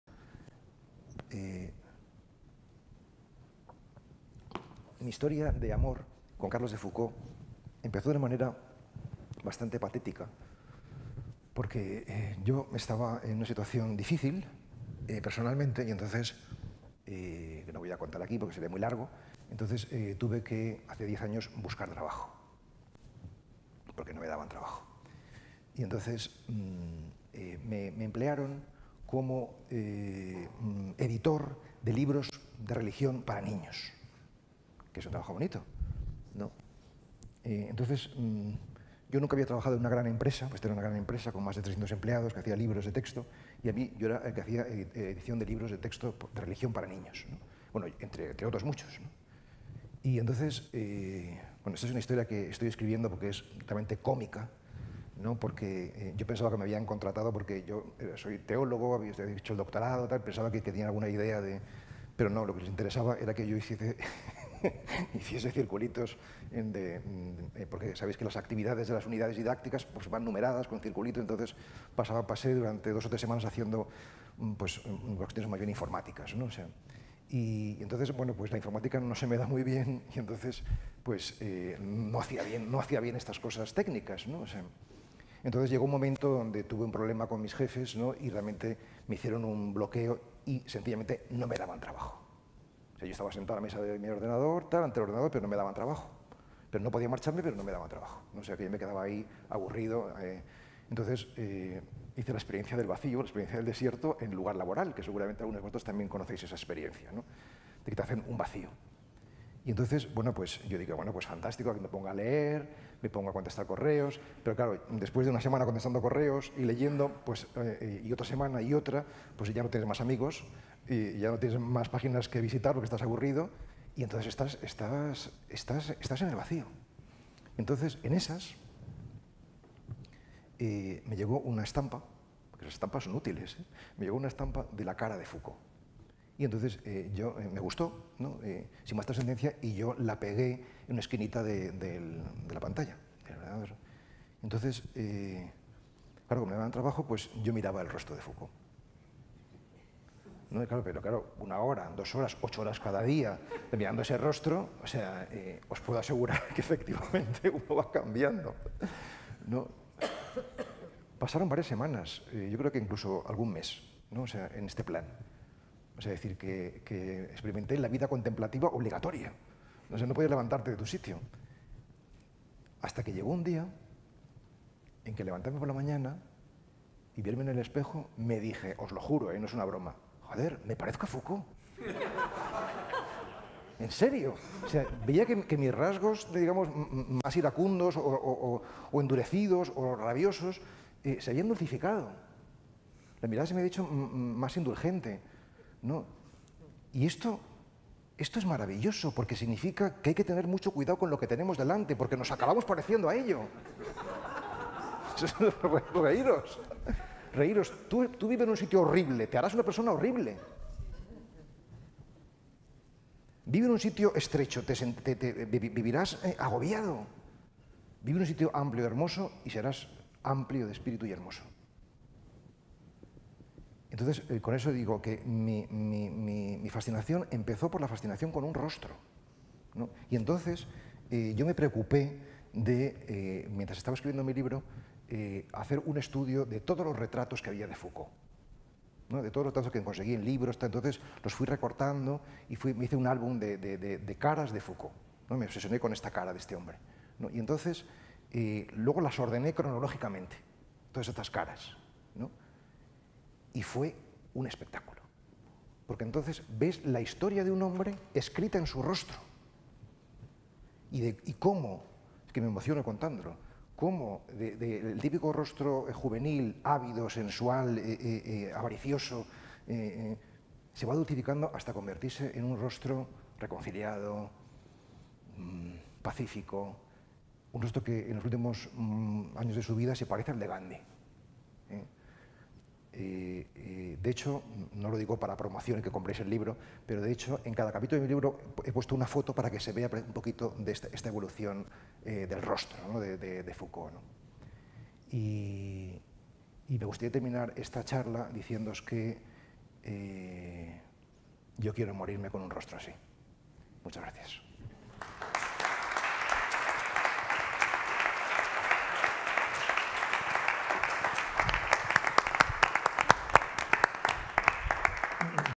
La Universalidad de la Práctica del Gurushakti
Relato de Pablo Dor's sobre su experiencia observando el rostro de Foucauld